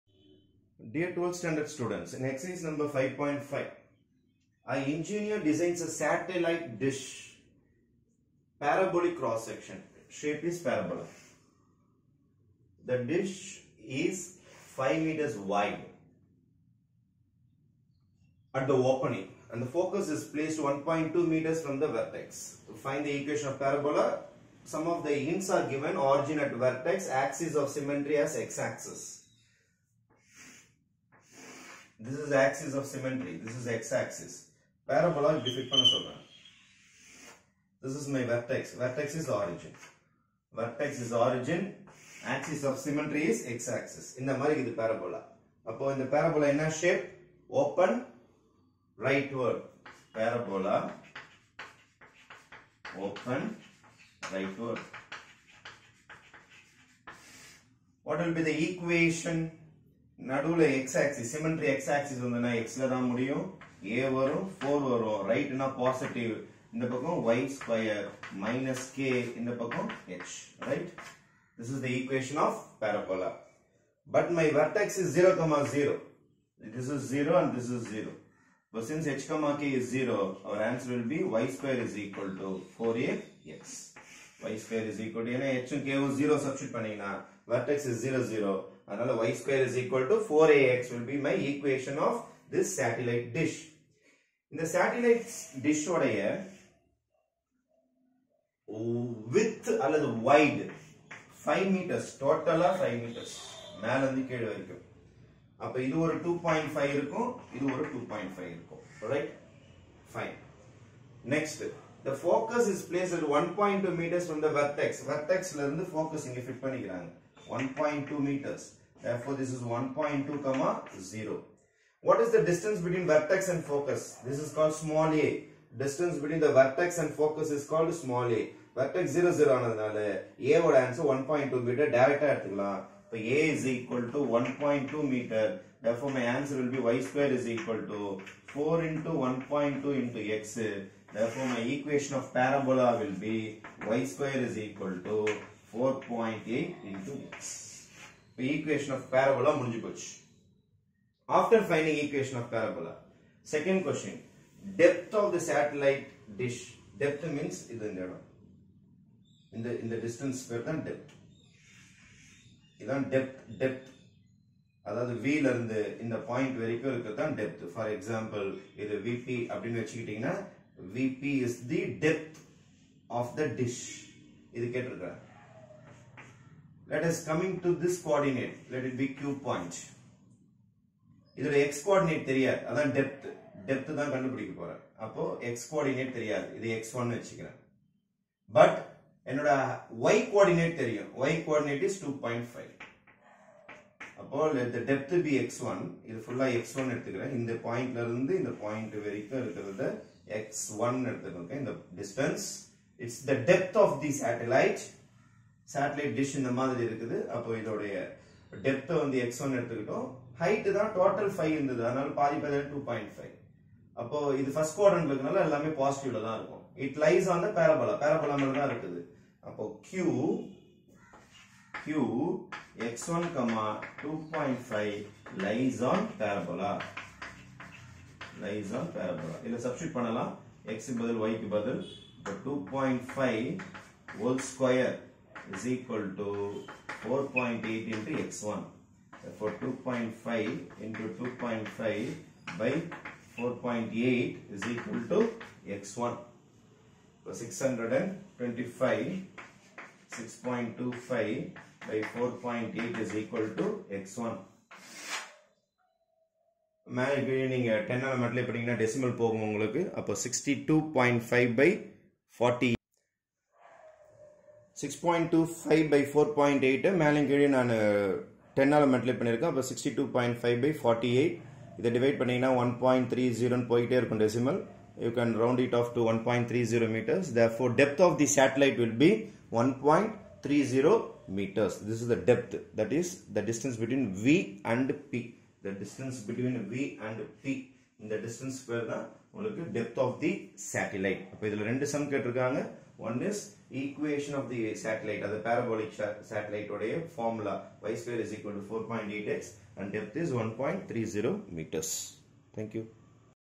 12th STD MATHS Ex.5.5(4) An engineer designs Parabolic satellite dish. Dish is 5m wide.....
TAMILNADU STATEBOARD XII MATHEMATICS